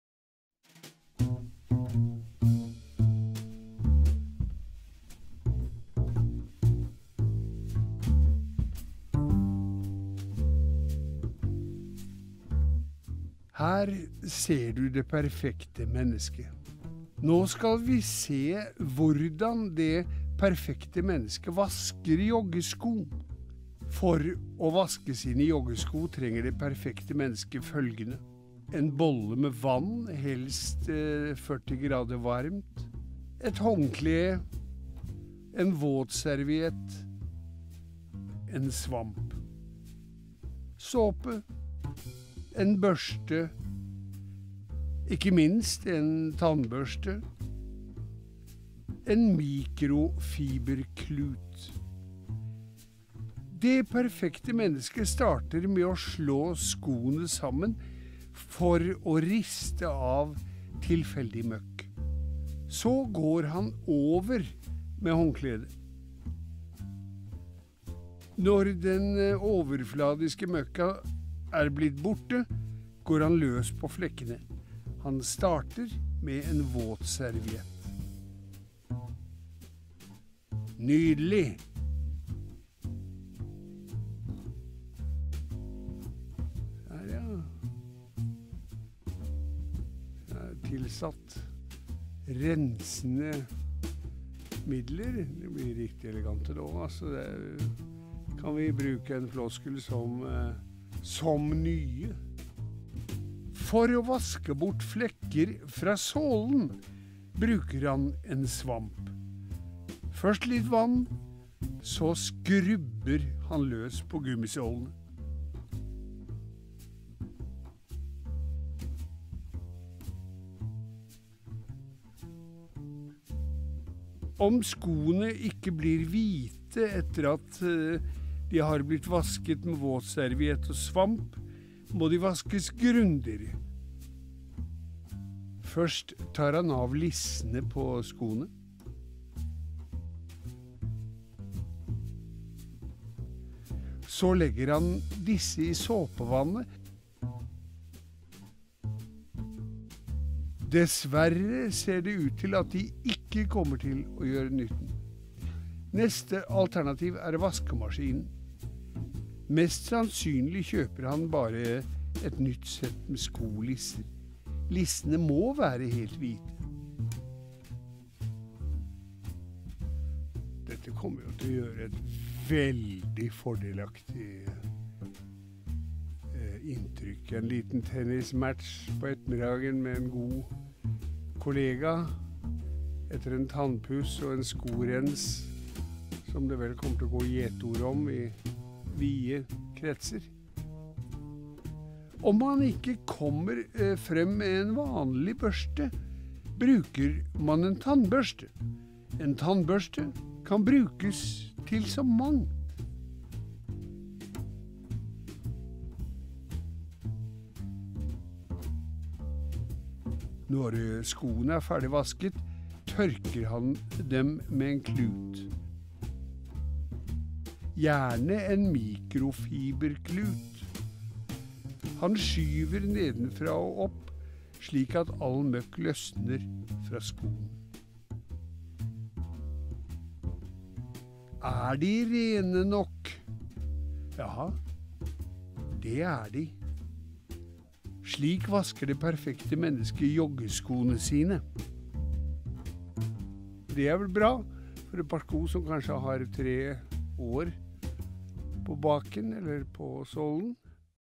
Paleet presenterer: Hvordan vaske joggesko
Det Perfekte Mennesket viser her hvordan du kan vaske dine joggesko. Med litt hjelp er det ikke så vanskelig. Ikke en gang hokus pokus.